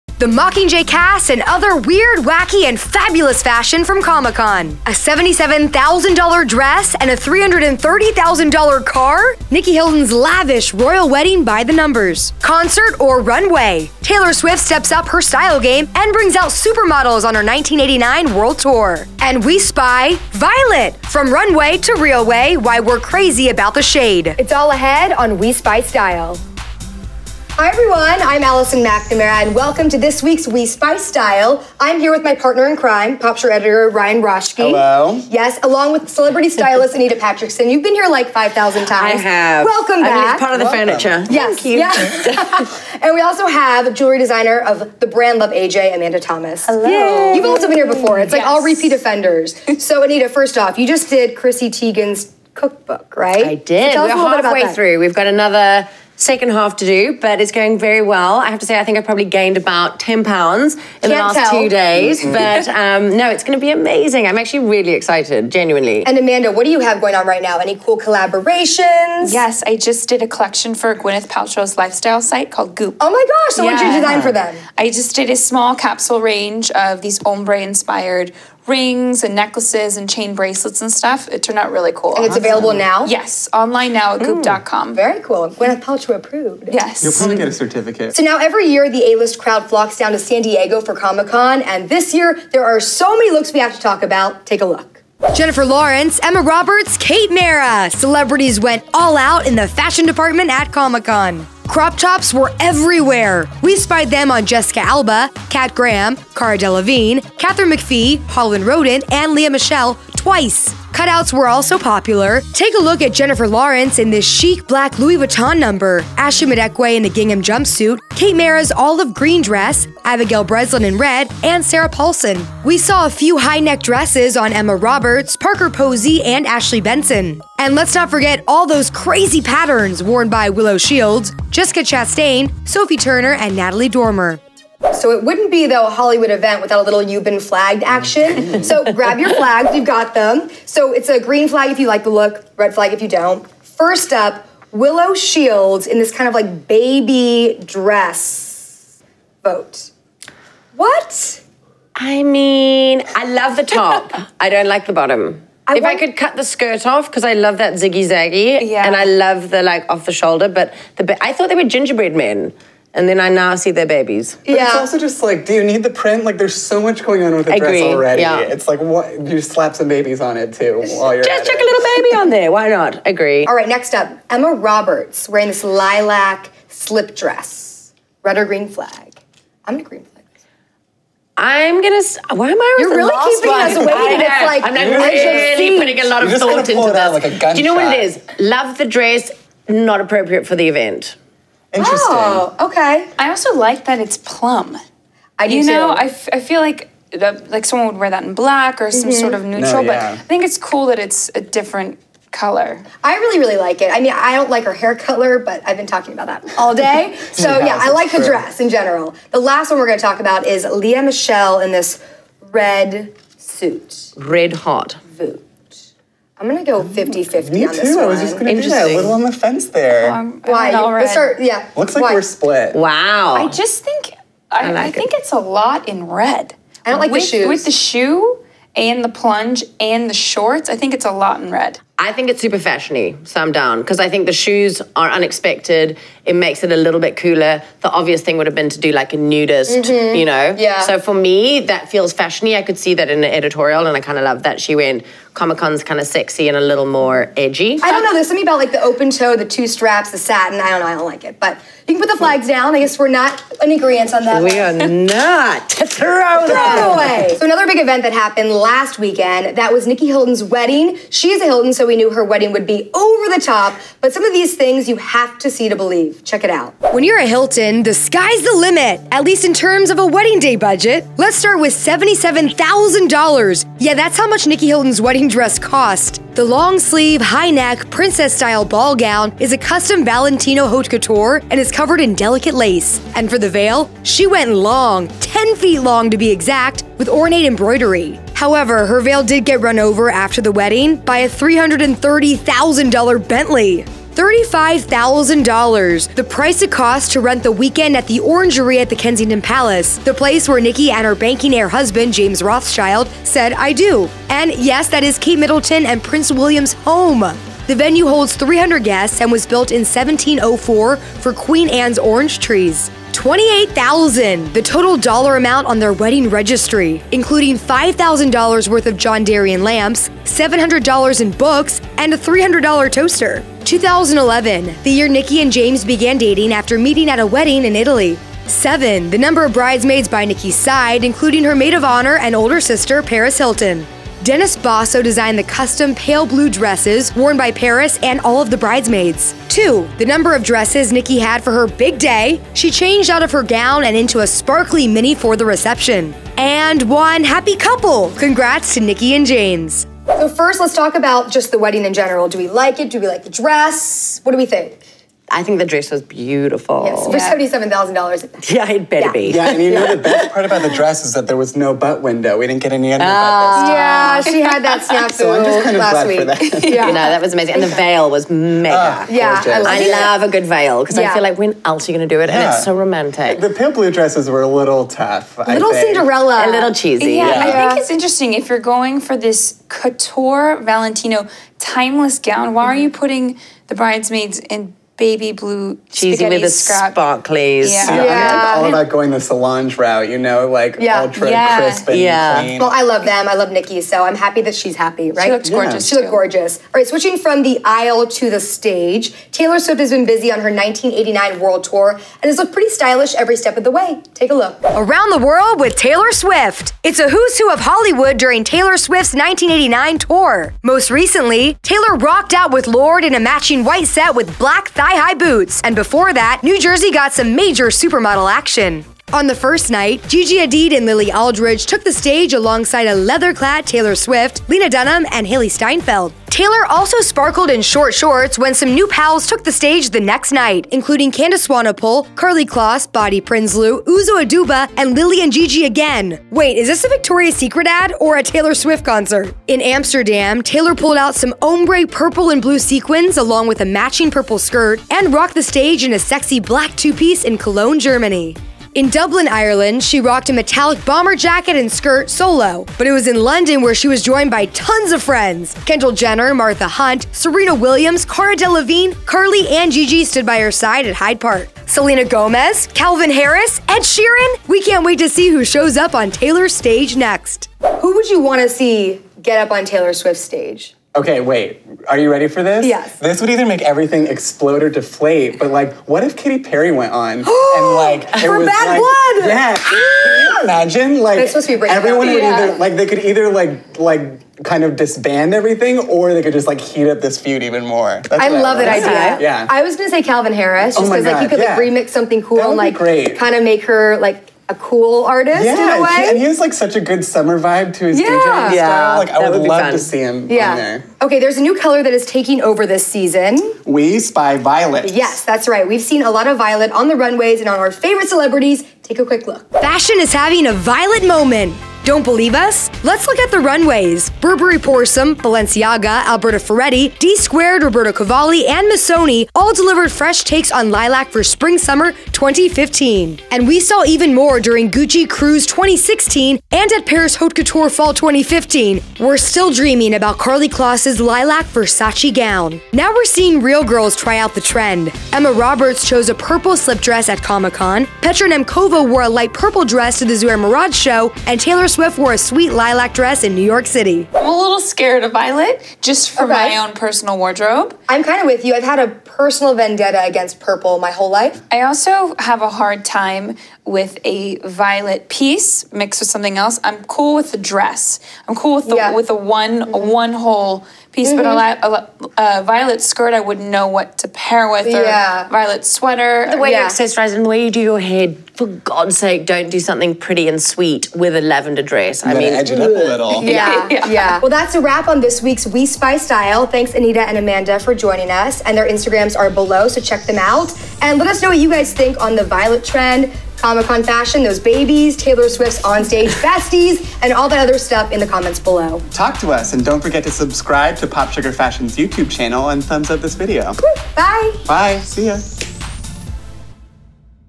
We Spy Style | The ONE Color You Have to Wear This Summer
Taylor Swift's 1989 concert tour outfits, Nicky Hilton's wedding, and Comic Con! It's a fun-filled week on We Spy Style with celebrity stylist Anita Patrickson and jewelry designer of Luv AJ, Amanda Thomas. Comic-Con delivered some major hits... and misses, including Willow Shields's questionable dress choice. And, we break down Nicky Hilton's lavish wedding by the numbers – her dress alone was worth $77,000! Watch on for all the scoop.

On Allison: Elizabeth and James dress, Stuart Weitzman shoes

We just got started on Vine!  Watch us there, too!